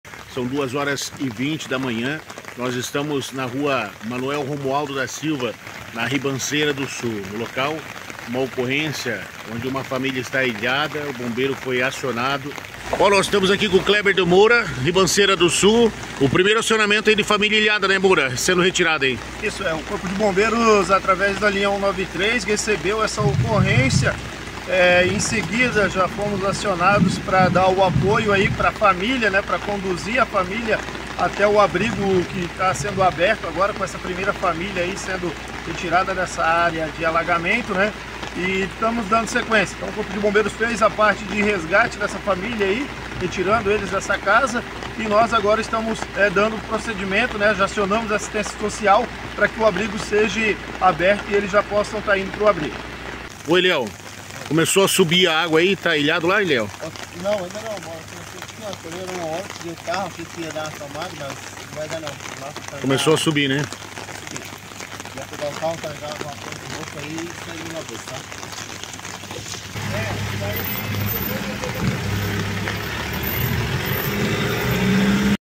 Enchente em São João Batista SC Família ilhada
matéria exclusiva acompanhe o canal madrugada desta sexta feira 17/11/2023